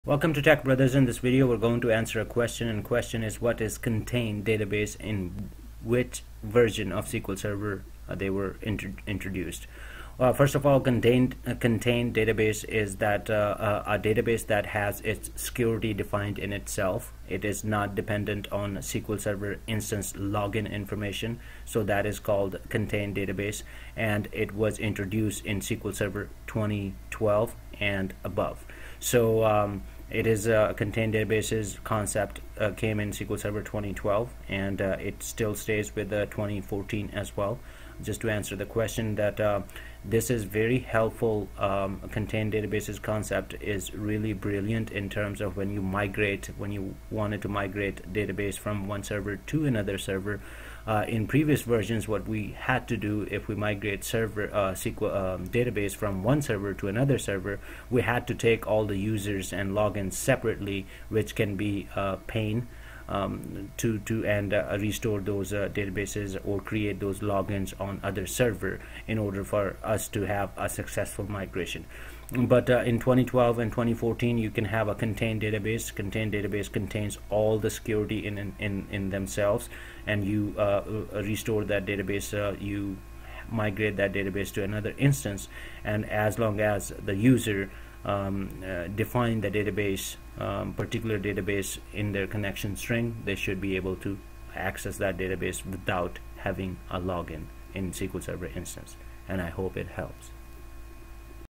What is Contained Database In which version of SQL SERVER they were introduced
SQL Server DBA Interview Question "What is Contained Database In which version of SQL SERVER  they were introduced"